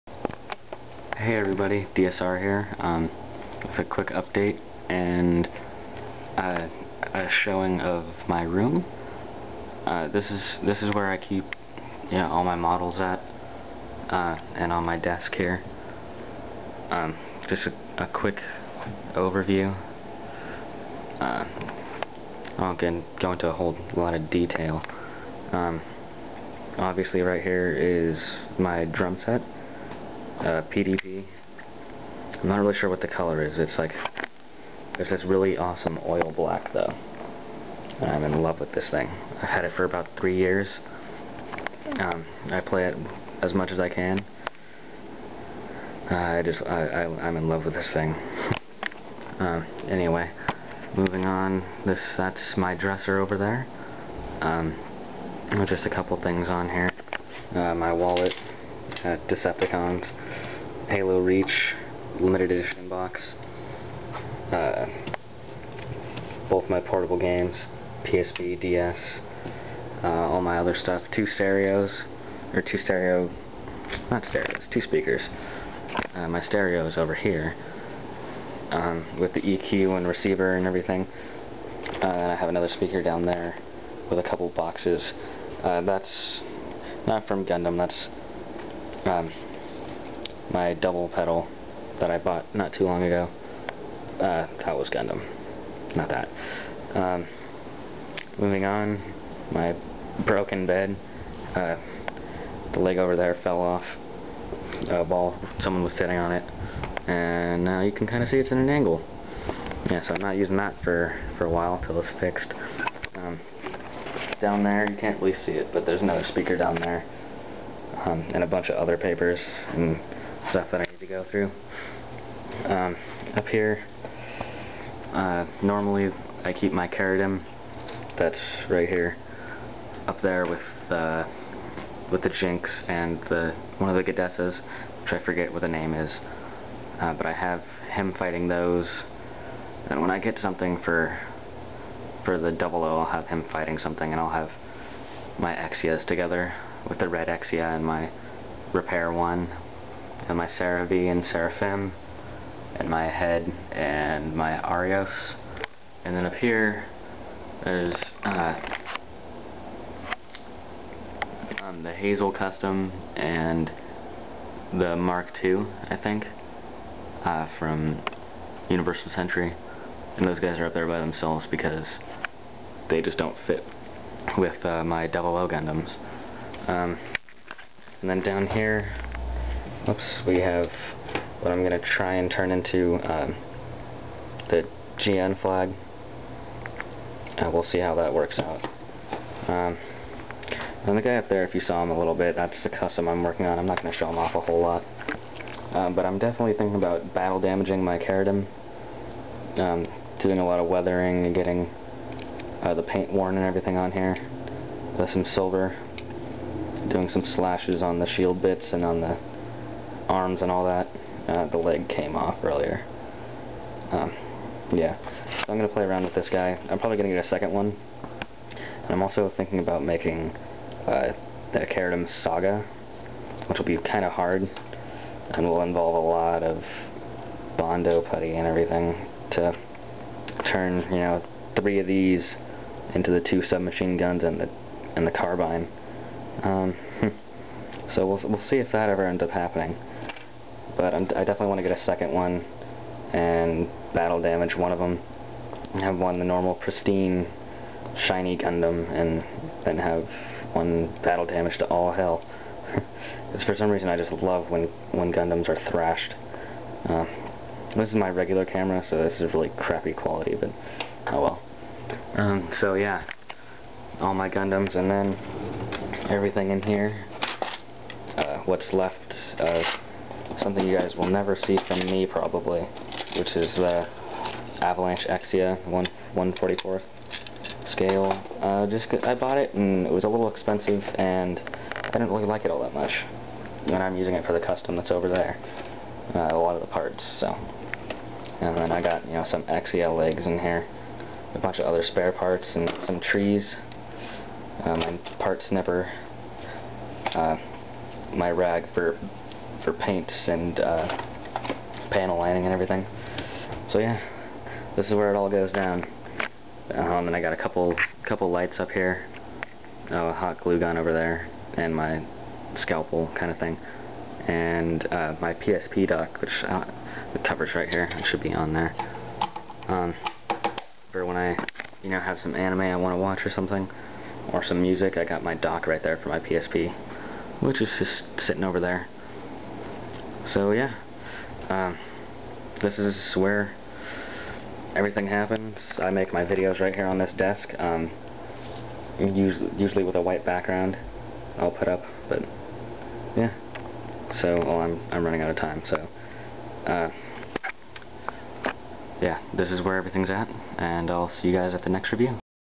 UPDATE: My Room and Gundams....
Not a whole lot of news, mostly showing everyone what everything looks like around my room, and showing off my models. I definately want to damage my Cherudim soon, so I suppose thats news... Also you can get a glimpse of my custom, unpainted and everything. Once it's painted, I'll make a video showing off everything it can do...